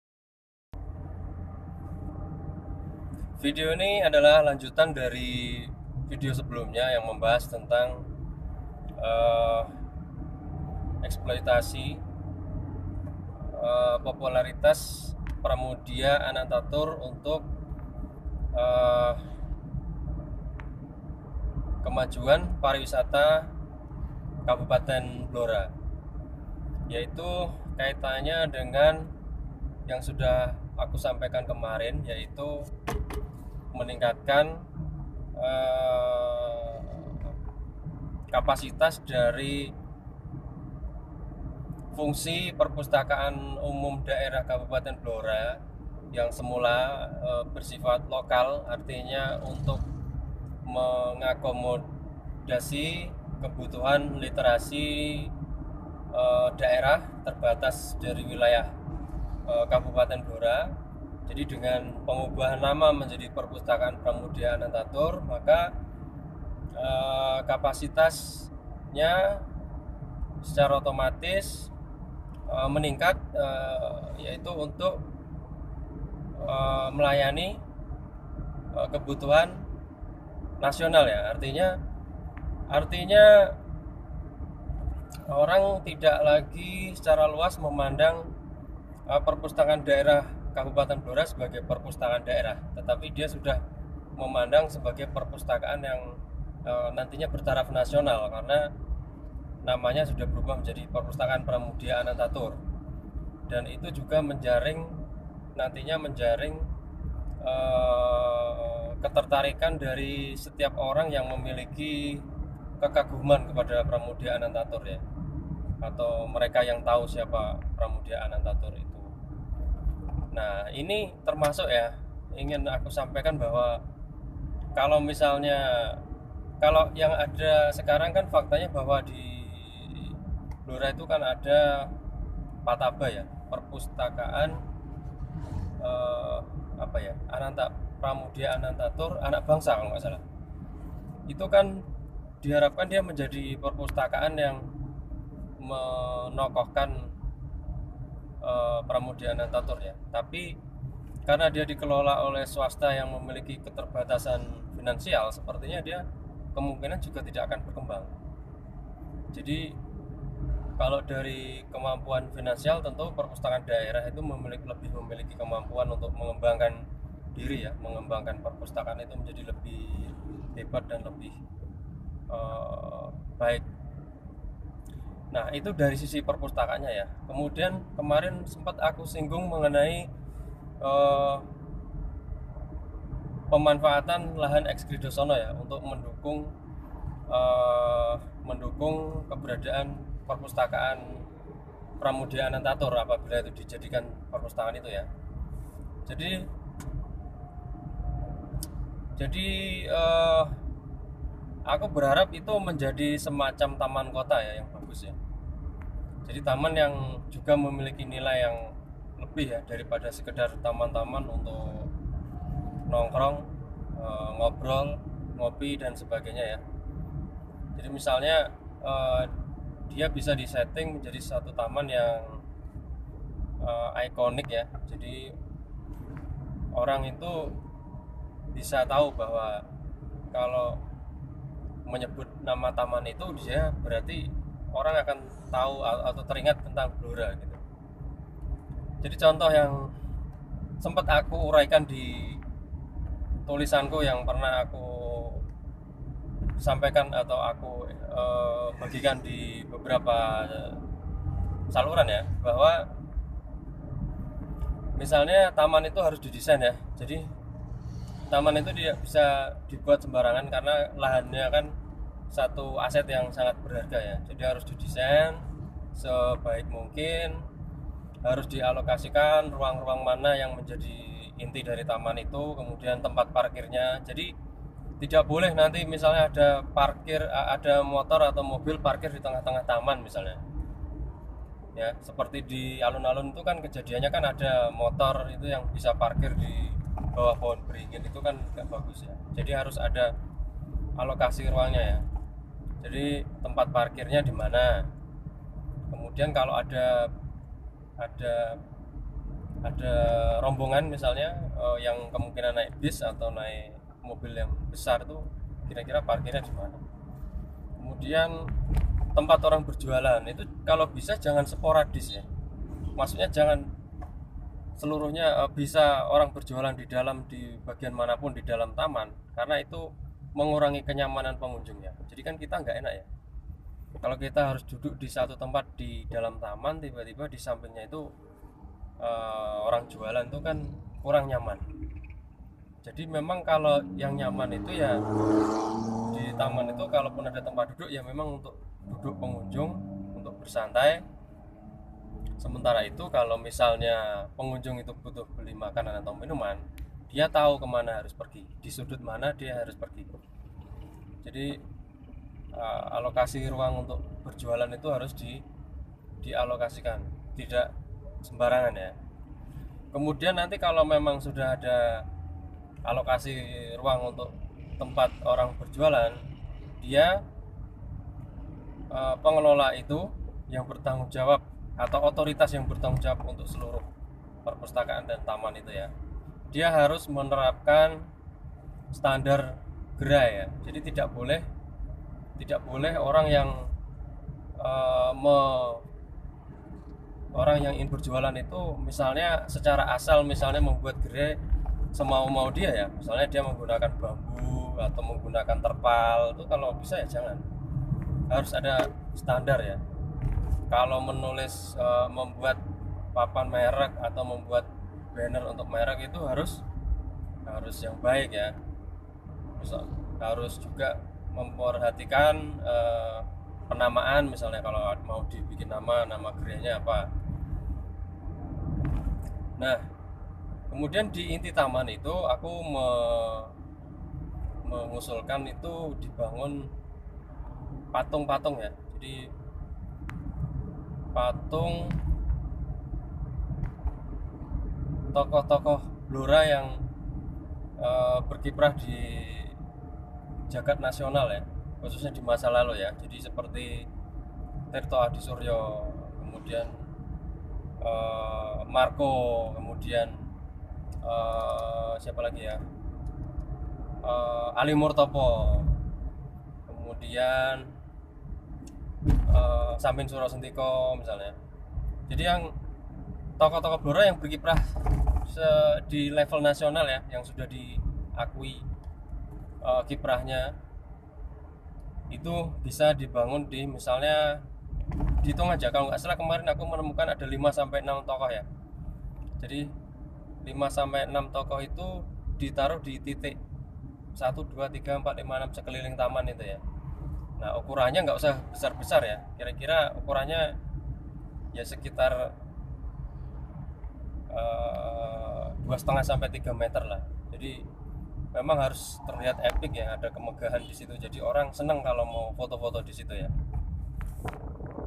Pramudya Ananta Toer untuk Blora 2
Enam tokoh Blora yang menasional :
1. Adi Sumarmo
2. Marco Kartodikromo
3. Tirto Adisuryo
4. Samin Surosentiko
5. Ali Moertopo
6. Pramudya Ananta Toer